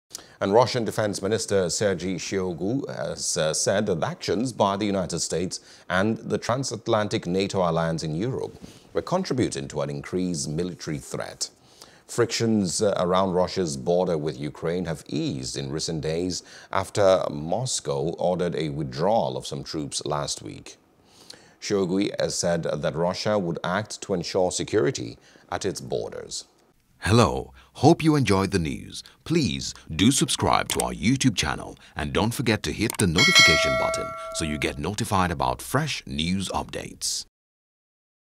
U.S., NATO Actions In Europe Increase Military Threat - Russia | FOREIGN
Russian Defence Minister Sergei Shoigu said  that actions by the United States and the transalantic NATO alliance in Europe were contributing to an increased military threat,.

Frictions around Russia's border with Ukraine have eased in recent days after Moscow ordered a withdrawal of some troops last week.